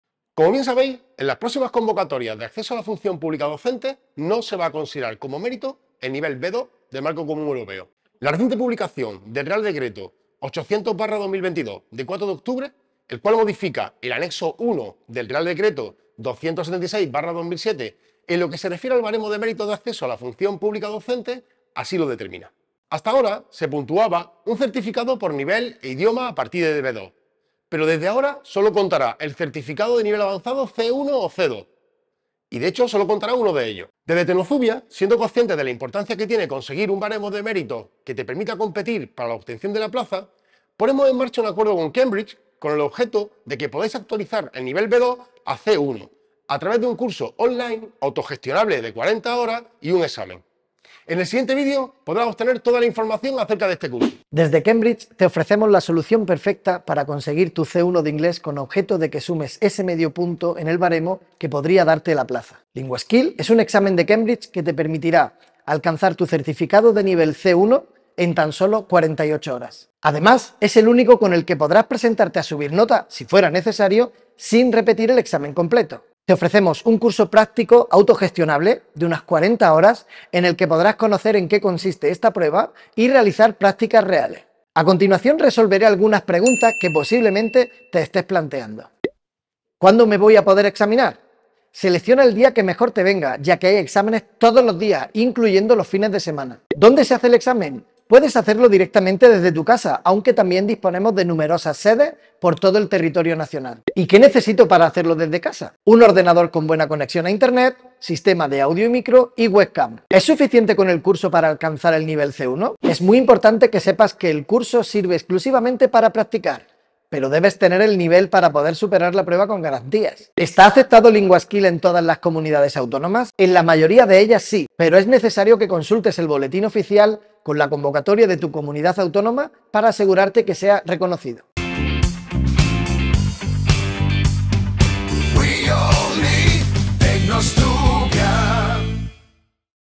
😱 ¡Ya no vale el B2 como mérito en las oposiciones de educación! Pero te damos una solución...
Tecnoszubia llega a un convenio con Cambridge con objeto de que puedas obtener tu certificado de nivel avanzado de inglés C1 a un menor precio. Además incluirá un curso para que puedas hacer prácticas antes de examinarte.